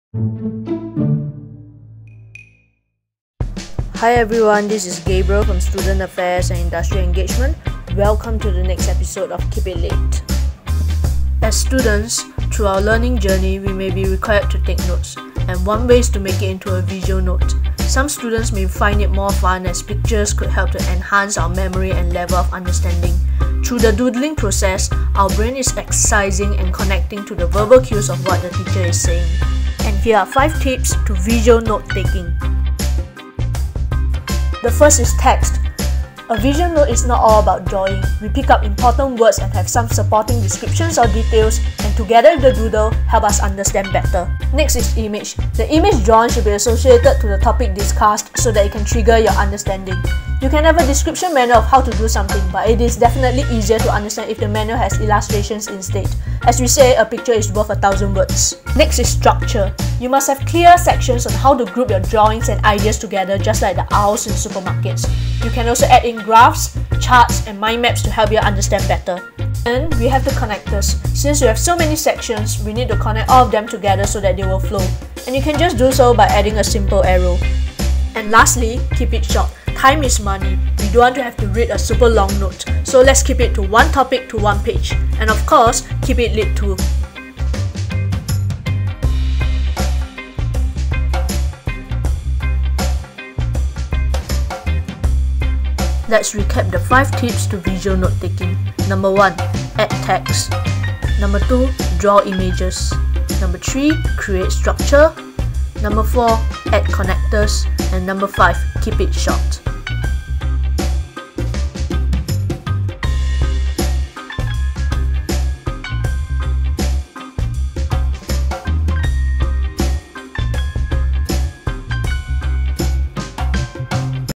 5 Tips to Visual Notetaking
While studying,  do you take notes? How about making notetaking a little more fun and perhaps easy to remember?

Click ▶️ and watch Keep it Lit - a series of videos on student development and wellness!